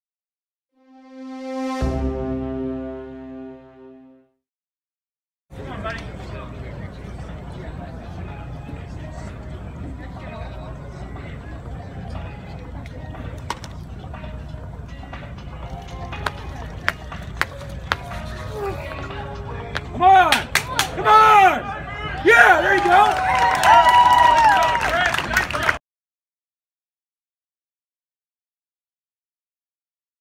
Oregon thrower Greg Skipper's fourth throw in NCAA hammer finals
Skipper logs a throw just over 70 meters